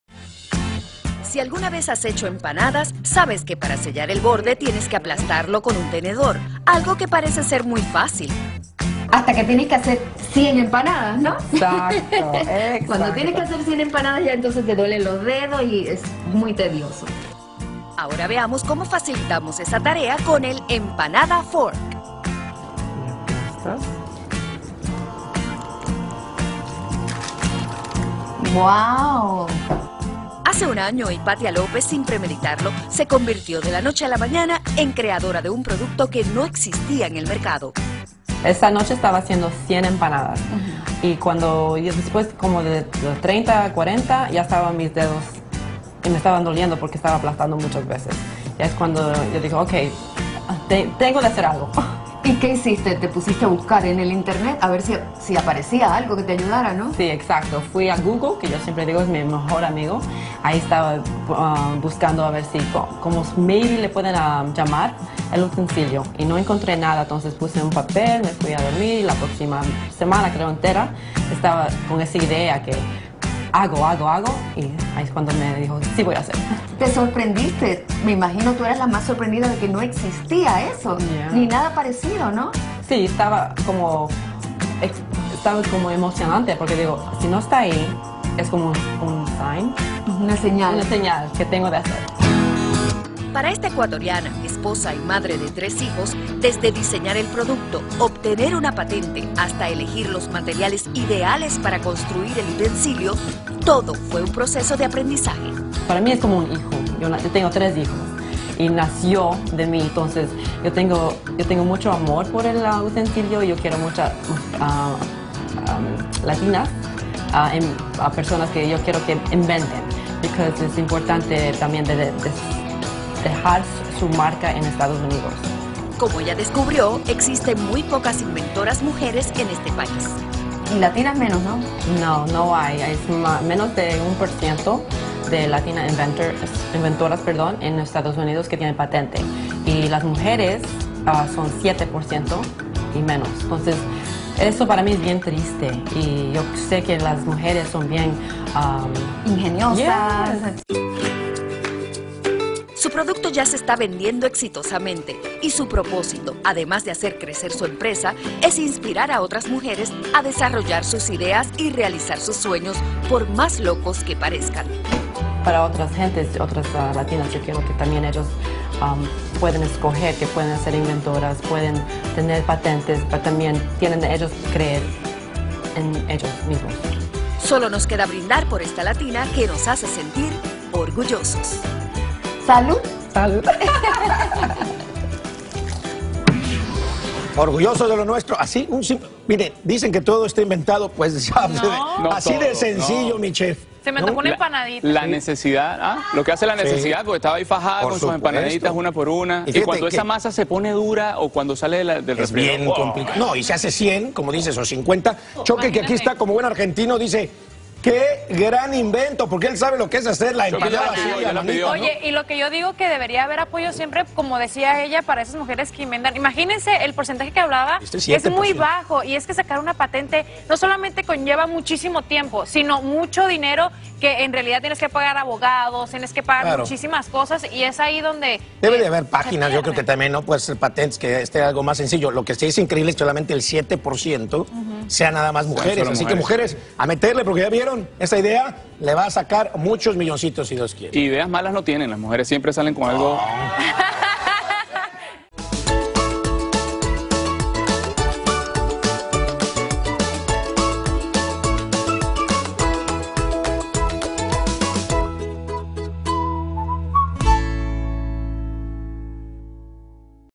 Hispana inventa este artículo de que facilita la vida al cocinar
Hipatia López es una madre hispana que, cansada de cerrar las empanadas una por una con tenedor, tuvo una brillante idea que la convirtió en inventora y empresaria a la vez: el "empanada fork". Esciucha su increíble historia.